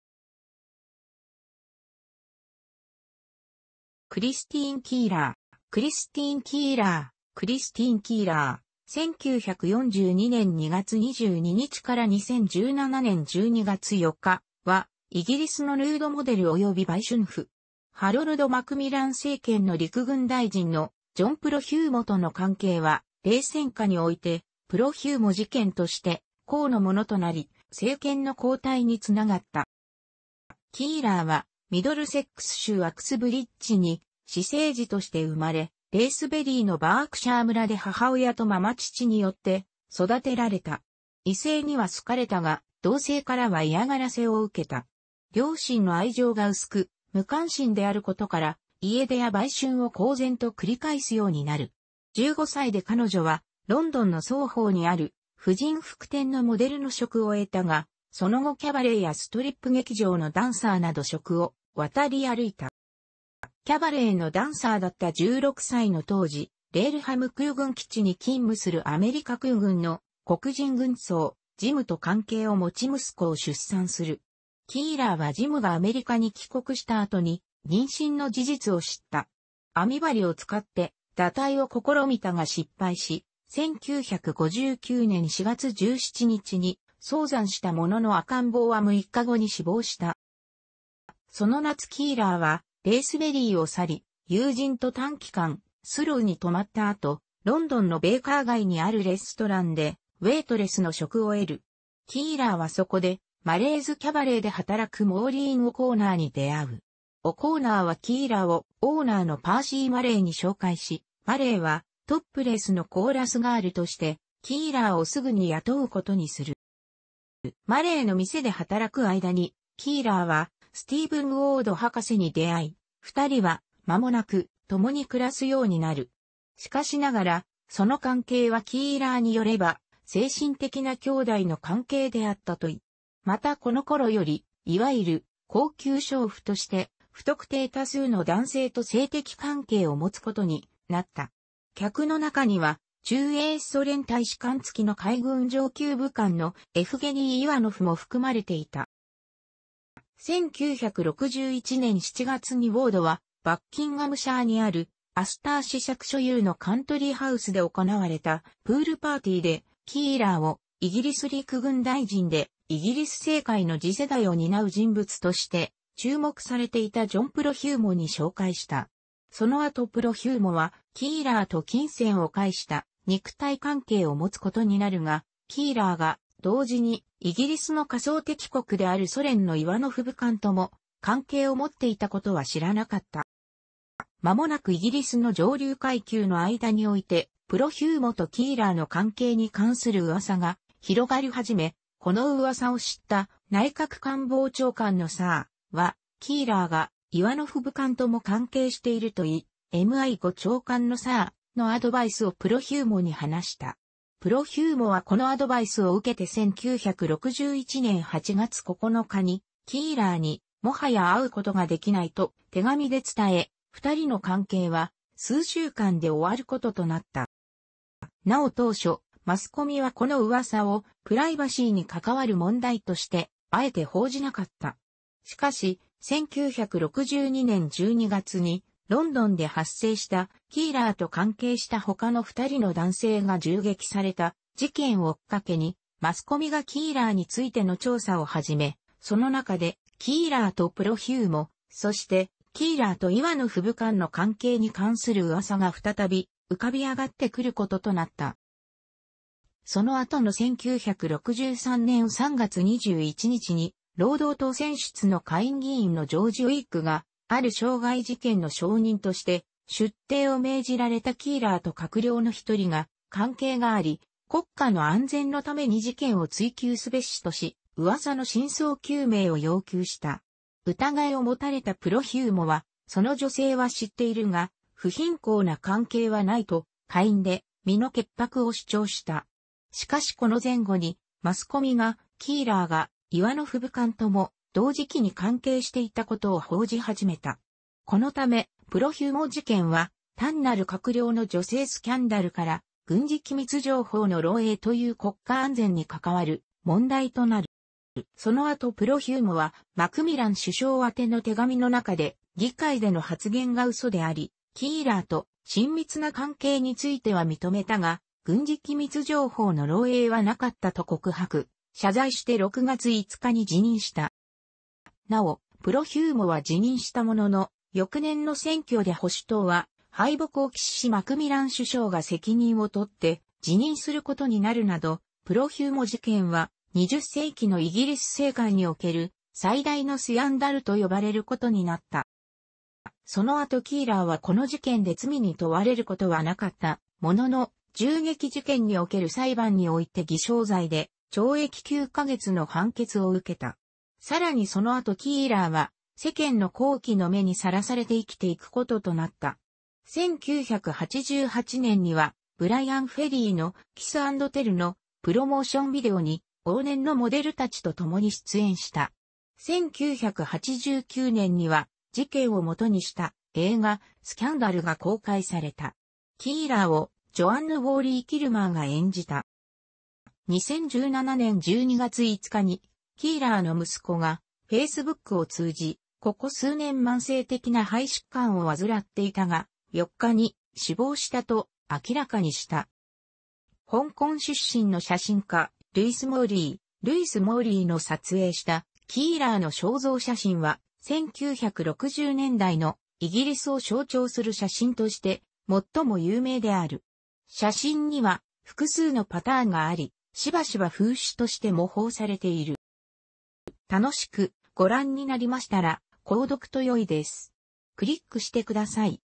クリスティーン・キーラー
クリスティーン・キーラー（Christine Keeler、1942年2月22日 - 2017年12月4日）は、イギリスのヌードモデルおよび売春婦。ハロルド・マクミラン政権の陸軍大臣のジョン・プロヒューモとの関係は、冷戦下において「プロヒューモ事件」として公の物となり、政権の交代につながった。

キーラーはミドルセックス州アクスブリッジに私生児として生まれ、レイスベリーのバークシャー村で母親と継父によって育てられた。異性には好かれたが、同性からは嫌がらせを受けた。両親の愛情が薄く無関心である事から、家出や売春を公然と繰り返すようになる。15歳で彼女はロンドンのソーホーにある婦人服店のモデルの職を得たが、その後キャバレーやストリップ劇場のダンサーなど職を渡り歩いた。

キャバレーのダンサーだった16歳の当時、レールハム空軍基地に勤務するアメリカ空軍の黒人軍曹「ジム」と関係を持ち息子を出産する。キーラーはジムがアメリカに帰国した後に妊娠の事実を知った。編み針を使って堕胎を試みたが失敗し、1959年4月17日に早産したものの赤ん坊は6日後に死亡した。

その夏キーラーはレイスベリーを去り、友人と短期間スロウに留まった後、ロンドンのベーカー街にあるレストランでウェイトレスの職を得る。キーラーはそこで「マレーズ・キャバレー」で働くモーリーン・オコーナーに出会う。オコーナーはキーラーをオーナーのパーシー・マレーに紹介し、マレーはトップレスのコーラスガールとしてキーラーをすぐに雇うことにする。

マレーの店で働く間に、キーラーはスティーヴン・ウォード博士に出会い、2人は間もなく共に暮らすようになる。しかしながらその関係はキーラーによれば「精神的な兄妹」の関係であったという。またこの頃よりいわゆる「高級娼婦」として不特定多数の男性と性的関係を持つことになった。客の中には駐英ソ連大使館付きの海軍上級武官のエフゲニー・イワノフも含まれていた。

1961年7月にウォードは、「（バッキンガムシャーにあるアスター子爵所有のカントリー・ハウス）」で行われた「プール・パーティー」で、キーラーをイギリス陸軍大臣で、「イギリス政界の次世代を担う人物」として注目されていたジョン・プロヒューモに紹介した。その後プロヒューモはキーラーと金銭を介した肉体関係を持つことになるが、キーラーが同時にイギリスの仮想敵国であるソ連のイワノフ武官とも関係を持っていたことは知らなかった。

間もなくイギリスの上流階級の間においてプロヒューモとキーラーの関係に関する噂が広がり始め、この噂を知った内閣官房長官のサー・は、「キーラーがイワノフ武官とも関係している」というMI5長官のサー・のアドバイスをプロヒューモに話した。プロヒューモはこのアドバイスを受けて1961年8月9日にキーラーに「もはや会うことができない」と手紙で伝え、2人の関係は数週間で終わることとなった。

なお当初、マスコミはこの噂を「プライバシーにかかわる問題」としてあえて報じなかった。しかし、1962年12月にロンドンで発生した、キーラーと関係した他の2人の男性が銃撃された事件をきっかけに、マスコミがキーラーについての調査を始め、その中でキーラーとプロヒューモ、そしてキーラーとイワノフ武官の関係に関する噂が再び浮かび上がって来ることとなった。

その後の1963年3月21日に、労働党選出の下院議員のジョージ・ウィッグが「ある傷害事件の証人として出廷を命じられたキーラーと閣僚の1人が関係があり、国家の安全のために事件を追及すべし」とし、噂の真相究明を要求した。疑いをもたれたプロヒューモは「その女性は知っているが、不品行な関係はない」と下院で身の潔白を主張した。しかしこの前後にマスコミが、キーラーがイワノフ武官とも同時期に関係していたことを報じ始めた。このため「プロヒューモ事件」は、単なる閣僚の女性スキャンダルから、「軍事機密情報の漏洩」という国家安全に関わる問題となる。

その後プロヒューモは、マクミラン首相宛の手紙の中で、「議会での発言が嘘であり、キーラーと親密な関係については認めたが、軍事機密情報の漏洩はなかった」と告白、謝罪して6月5日に辞任した。

なお、プロヒューモは辞任したものの、翌年の選挙で保守党は敗北を喫しマクミラン首相が責任を取って辞任することになるなど、プロヒューモ事件は「20世紀のイギリス政界における最大のス...